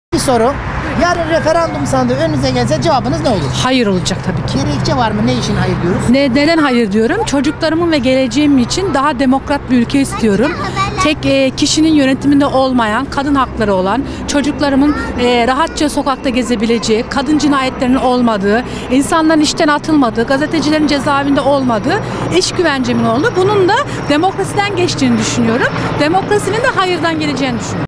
Hayır'ın nedenleri saymakla bitmez
Neden mi hayır? Çocuklarım ve geleceğim için daha demokrat bir ülke istiyorum. Tek kişinin yönetiminde olmayan, kadın hakları olan, çocuklarımın rahatça sokakta gezebileceği, kadın cinayetlerinin olmadığı , insanların işten atılmadığı, gazetecilerin cezaevinde olmadığı, iş güvencemin olduğu bir ülke istiyorum. Bunun da demokrasiden geçtiğini düşünüyorum. Demokrasinin de Hayır'dan geleceğini düşünüyorum.